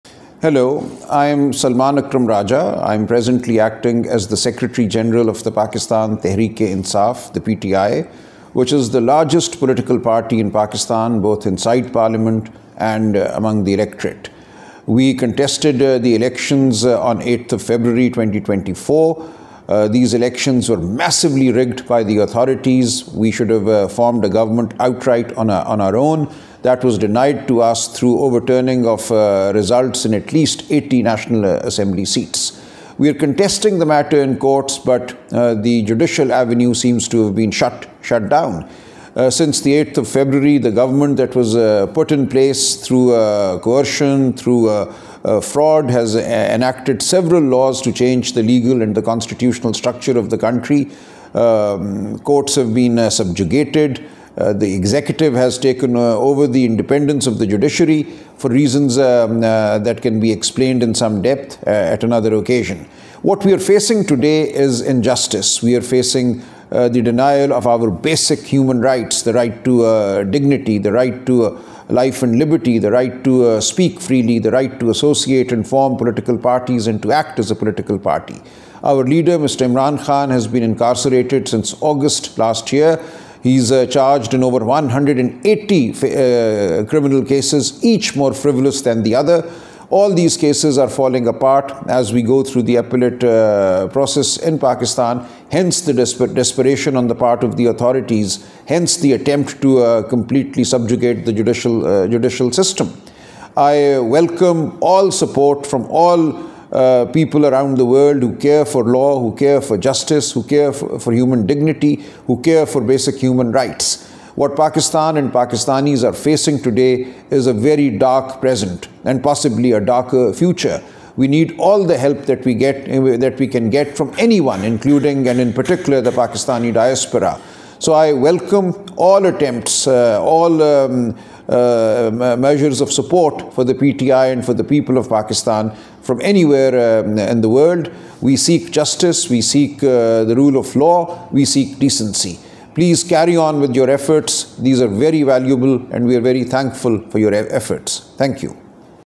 Secretary General PTI Mr. Salman Akram Raja’s message for overseas Pakistanis
Secretary General PTI Mr. Salman Akram Raja’s message for overseas Pakistanis, urging them to unite and stand against unconstitutional and illegal actions taking place in Pakistan.

He calls on the Pakistani diaspora to lend their voices, through peaceful and organized protests, in solidarity with the cause of justice and democracy.

A peaceful protest against illegal and politically motivated incarceration of Former PM Imran Khan, PTI Leadership, and party workers and supporters is organised in London tomorrow: